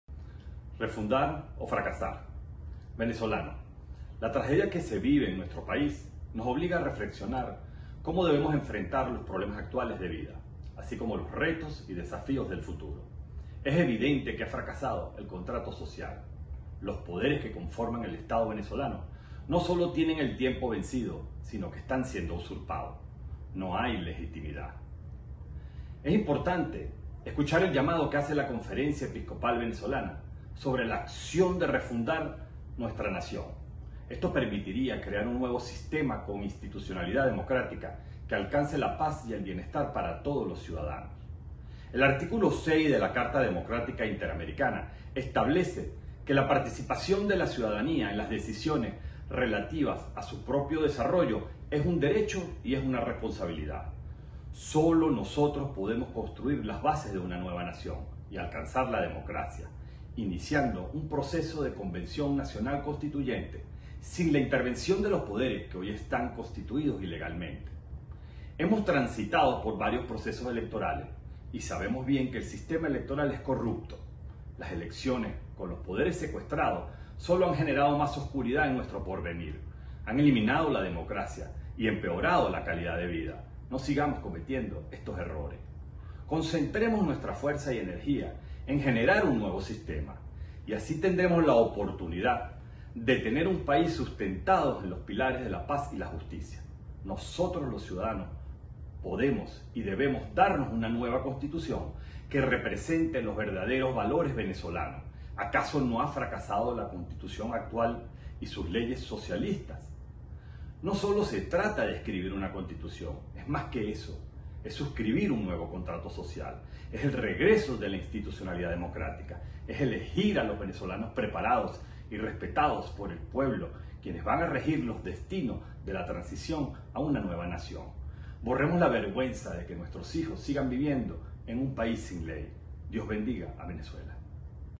Exmagistrado Miguel Ángel Martín propone una constituyente contra Maduro
En su mensaje a los asistentes del foro “Refundación Nacional Constituyente”, Martín dijo que la tragedia política, económica y social, tras 22 años de gobierno chavista, obliga a los venezolanos a reflexionar cómo enfrentar los desafíos del futuro.